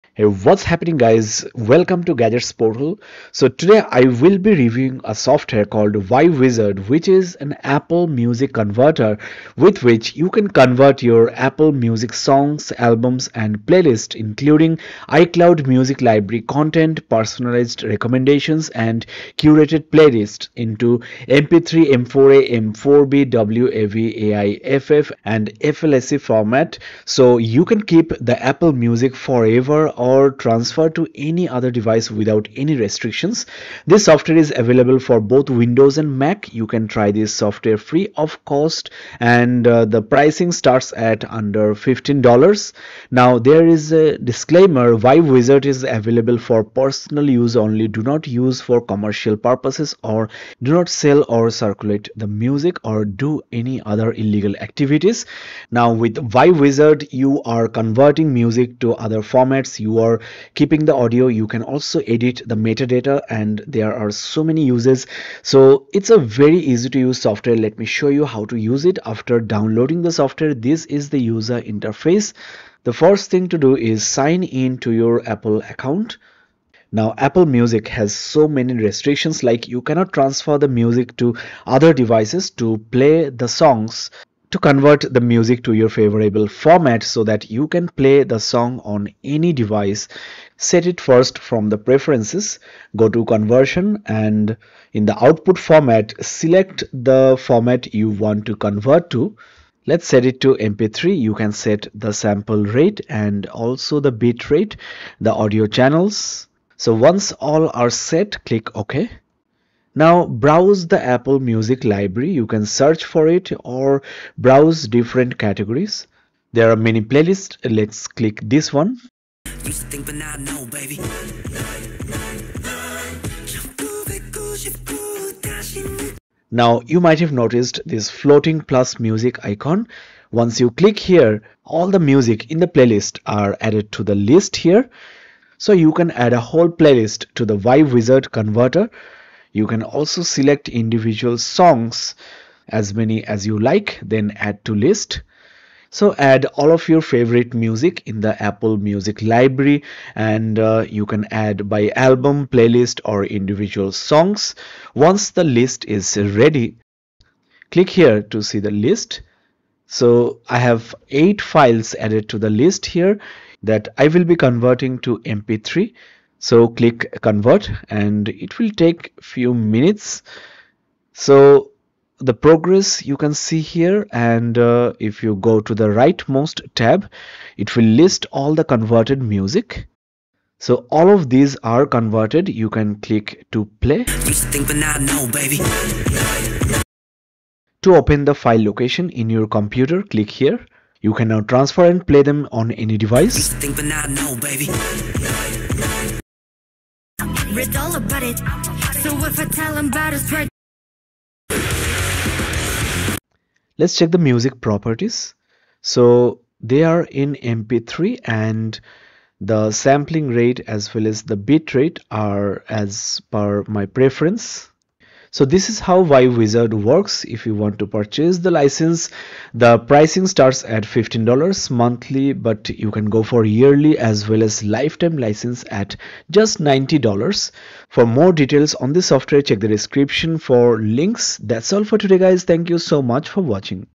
How to Convert Apple Music to MP3, M4A without Restrictions?
ViWizard is a full-featured Apple Music converter that easily converts Apple Music songs, albums, playlists—including personalized recommendations, and curated playlists—into MP3, M4A, M4B, WAV, AIFF, and FLAC so you can keep your Apple Music forever or transfer it to any device without restrictions.